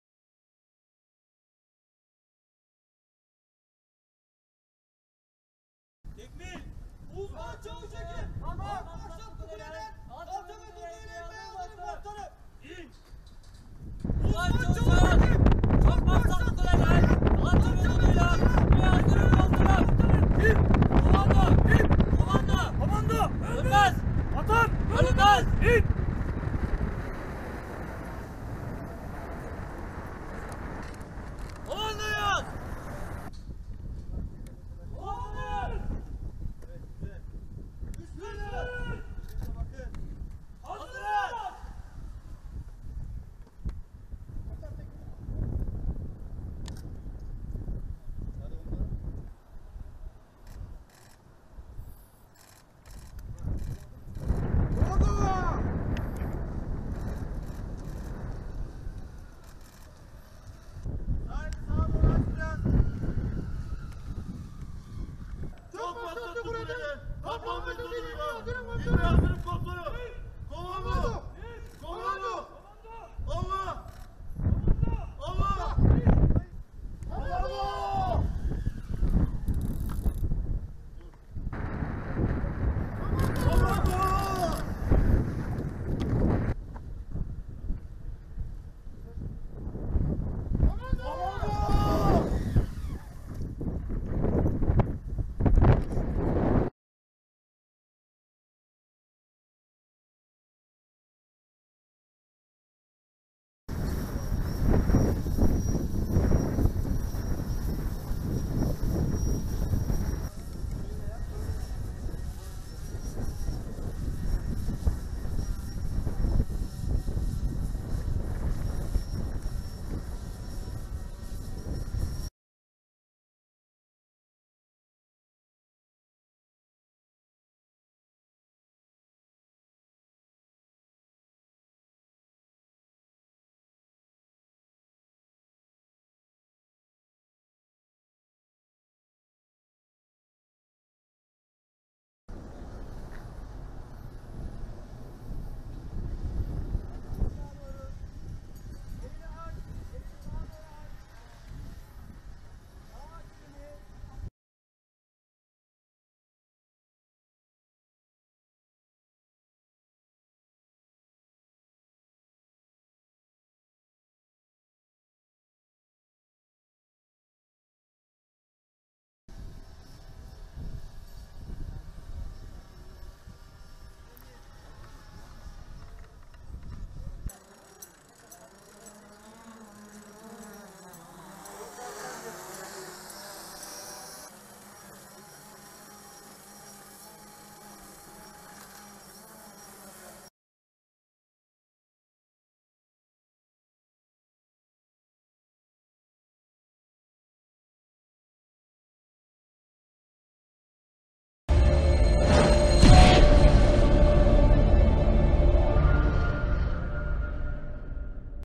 |HD| Zorlu Türk Komando Eğitimleri 2019 | Turkish Commando Trainings
Zorlu komando eğitimlerini gösteren bir video.